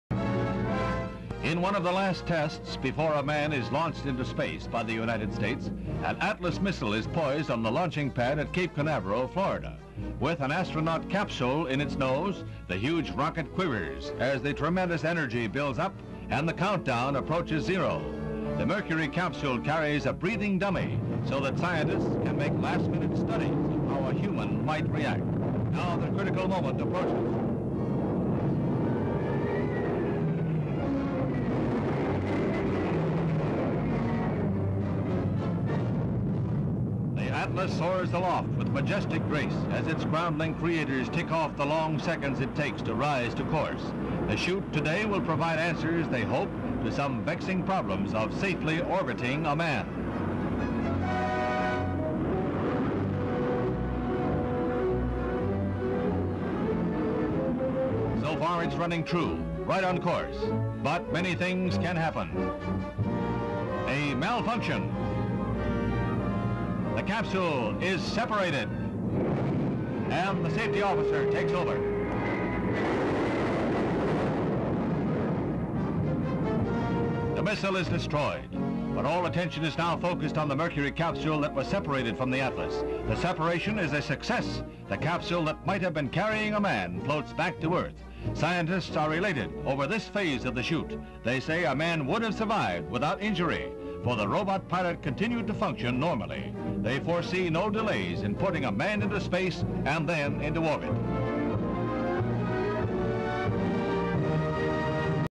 Atlas Failure and Success (1961)
An Atlas missile explodes after launch but the test capsule, which would carry a human, survived the explosion.